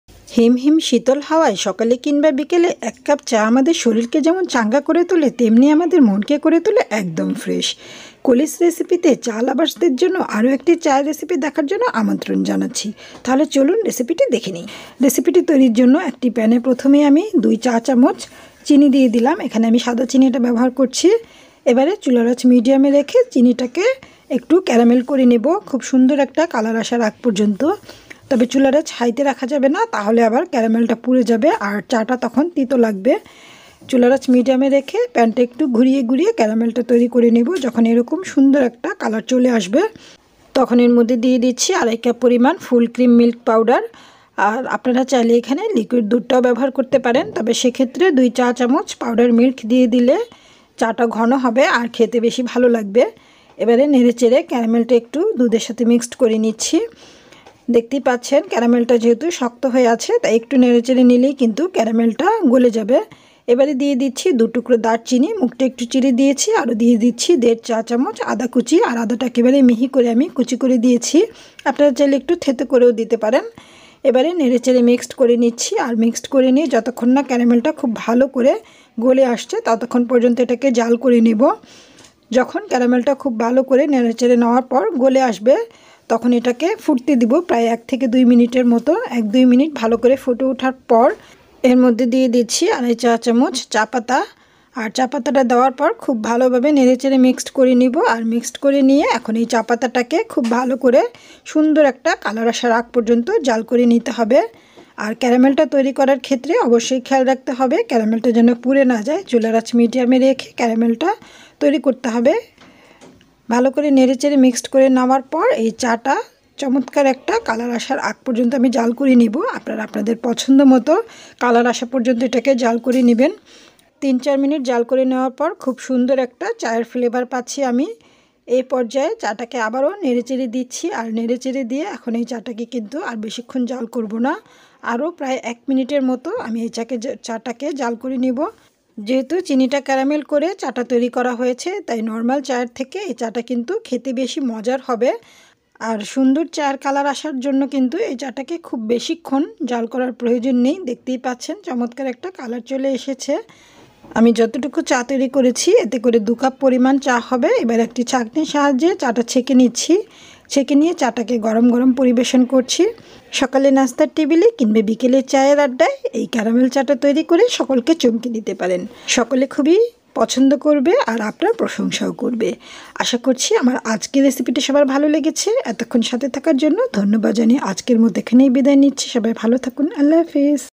বিকেলে চায়ের আড্ডায় এক কাপ ক্যারামেল চা || Caramel tea in the morning or evening for a fresh mind
সহজ রেসিপিতে দারুণ স্বাদের ক্যারামেল চা।খুব সহজে ও অল্প সময়ে তৈরি। আশা করছি আমার আজকের রেসিপিটি সবার ভালো লাগবে।
সবাইকে অসংখ্য ধন্যবাদ।